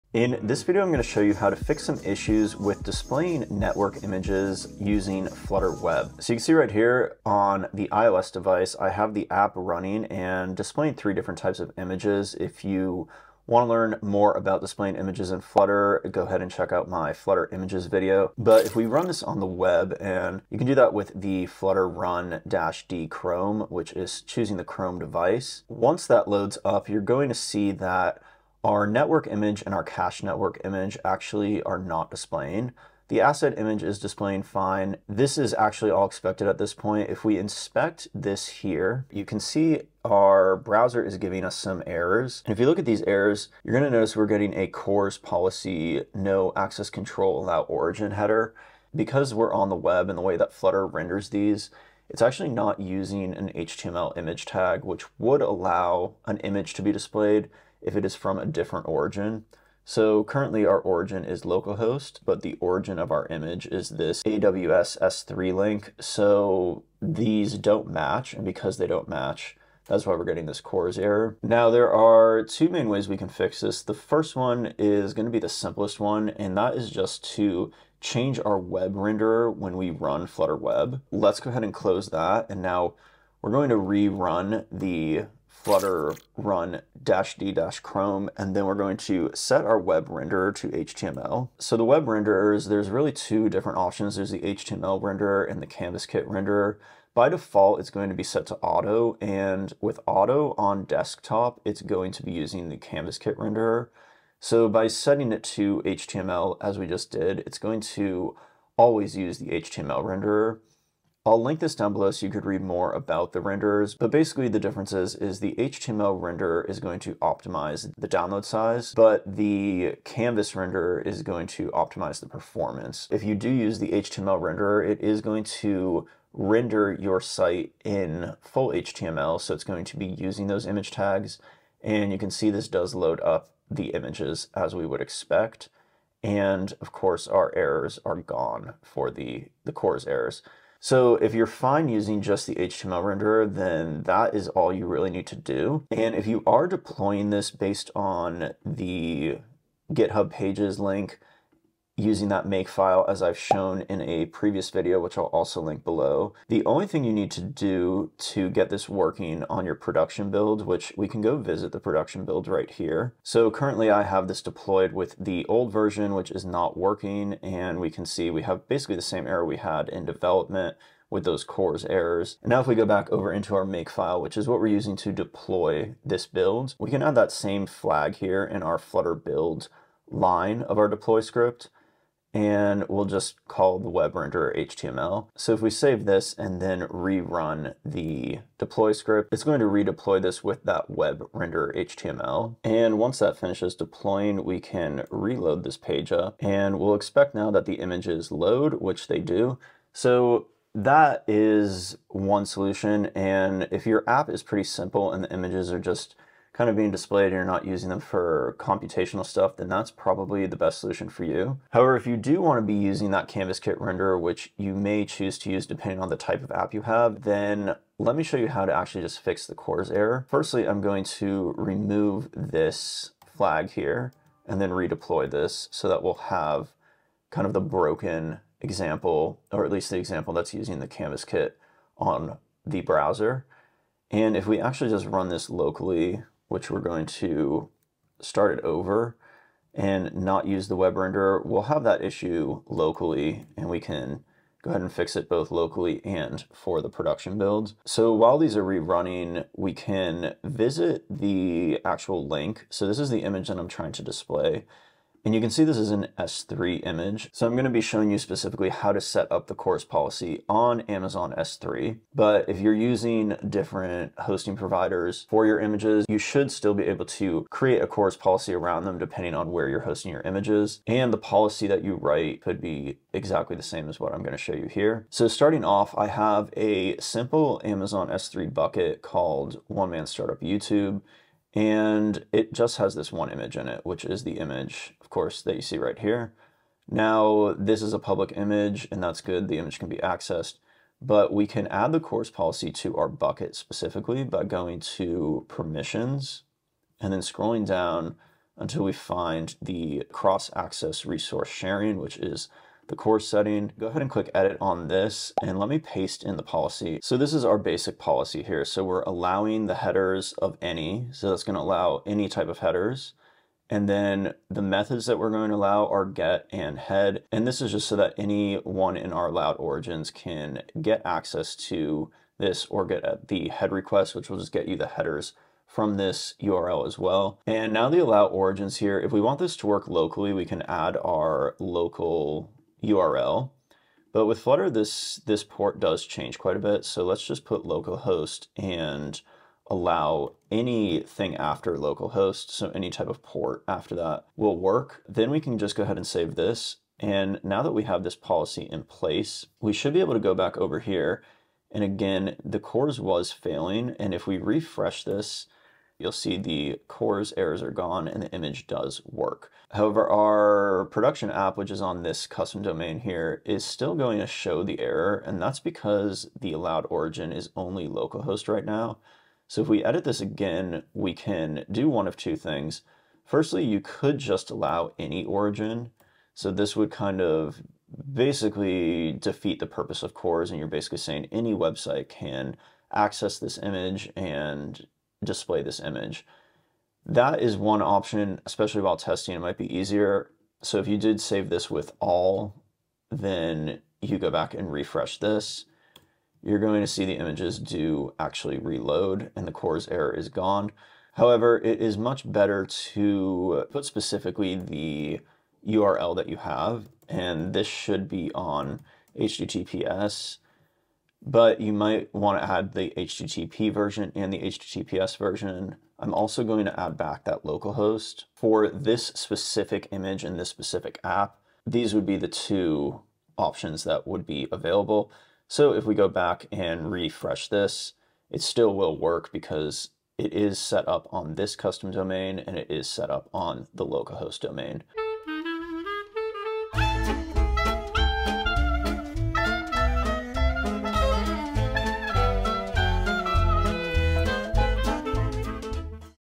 Flutter Web Images: Fix Network Image Not Loading
Learn two different ways you can fix network images not loading for flutter web. The first way is to use the web renderer of HTML, this will render you images in an HTML image tag which will avoid any CORS issues. If you'd rather not use the HTML renderer then you'll also see how you can update the CORS settings for an AWS S3 bucket to satisfy the Allowed Origin Header.

SECTIONS
0:00 - The Problem
0:25 - Example Of CORS Error
1:11 - Option 1: Using Flutter's HTML Web Renderer
2:25 - Updating Deploy Script
3:31 - Option 2: Configure CORS
4:35 - Updating CORS In AWS S3